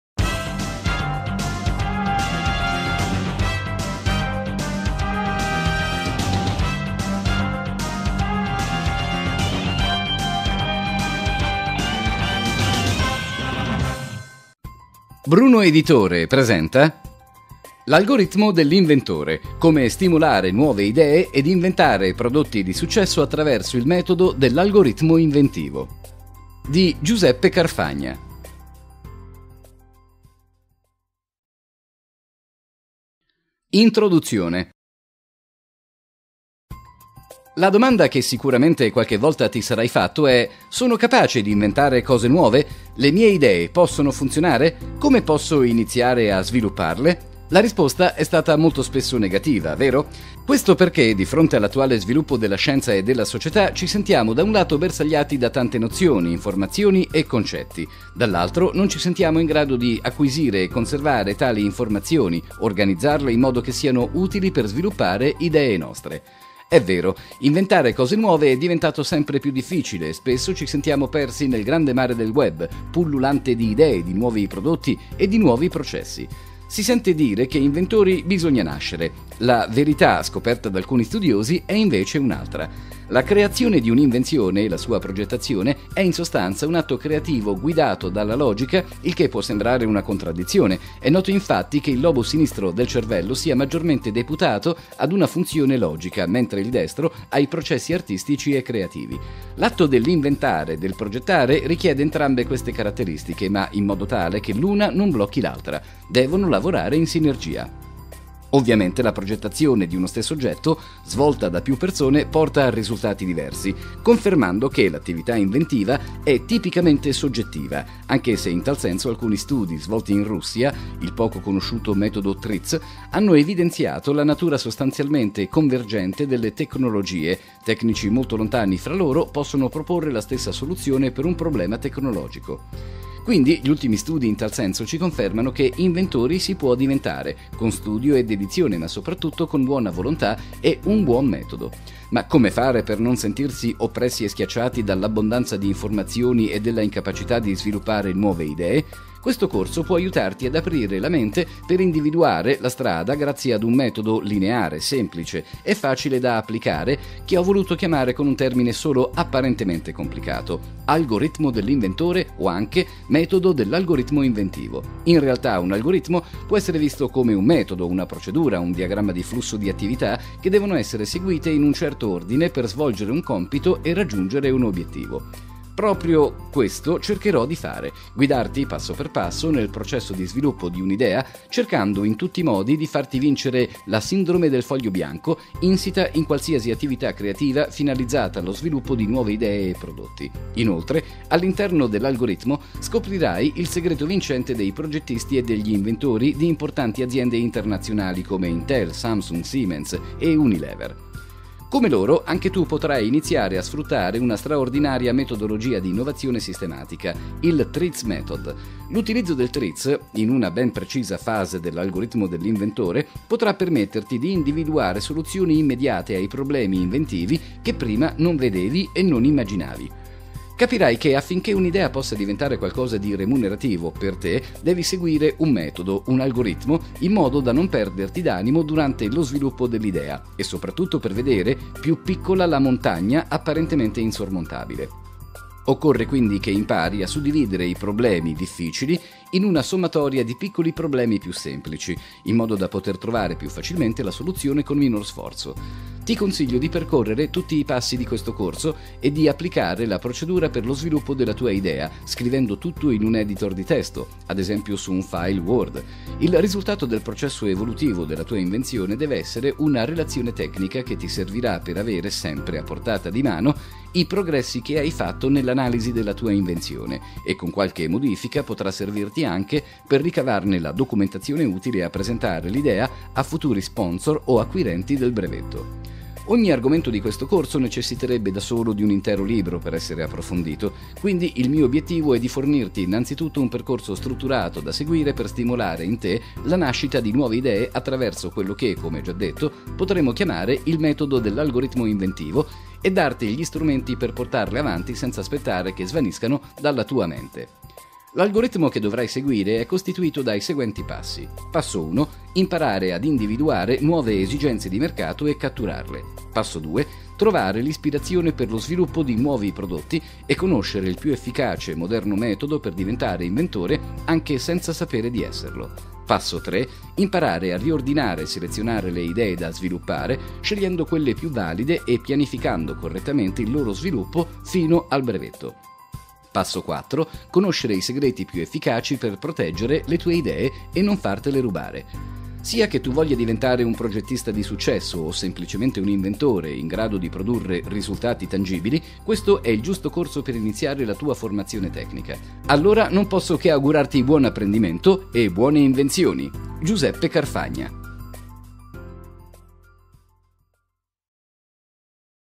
L'Algoritmo dell'Inventore: Videocorso Pratico
Come Stimolare Nuove Idee e Inventare Prodotti di Successo attraverso il Metodo dell'Algoritmo Inventivo -VideoEbook-Bruno Editore
Autore - Giuseppe Carfagna

Programma di L'Algoritmo dell'Inventore

COME INDIVIDUARE LE NUOVE ESIGENZE

 ✓ In che modo si può essere creativi anche senza avere talento.
 ✓ Come migliorare i propri risultati con il principio 80/20.
 ✓ Come far aumentare le probabilità di avere un'intuizione.
 ✓ Come trovare l'ispirazione in quello che hanno fatto altri prima di noi.
 ✓ Cos'è e come si fa un'analisi di anteriorità brevettuale.
 ✓ Cos'è Espacenet e perché è così utile.

COME POTENZIARE LA TUA CREATIVITA'

 ✓ Come riconoscere un carattere creativo.
 ✓ In che modo le mappe mentali possono aiutarti nel processo di sviluppo creativo.
 ✓ In che modo si può stimolare la creatività.
 ✓ Cos'è e come funziona il metodo TRIZ.

COME RIORDINARE LE TUE IDEE E FORMALIZZARLE

 ✓ Come evitare di sabotarsi portando avanti solo le idee profittevoli.
 ✓ Come funziona il Metodo della Matrice delle Scelte e in che modo può aiutarti a scegliere l'idea più produttiva.
 ✓ In che modo un software CAD/CAE può aiutarti a migliorare la presentazione e la realizzazione della tua idea.
 ✓ Come creare un gruppo di persone affiatate e specializzate per raggiungere più velocemente i tuoi traguardi.
 ✓ Come utilizzare il metodo di Gantt per pianificare il tuo progetto.

COME PROTEGGERE LE TUE IDEE

 ✓ Quali sono le precauzioni da prendere per proteggere la propria idea.
 ✓ Come comportarsi e a chi rivolgersi in caso di contenzioso.
 ✓ Come preparare la domanda di brevetto per via telematica.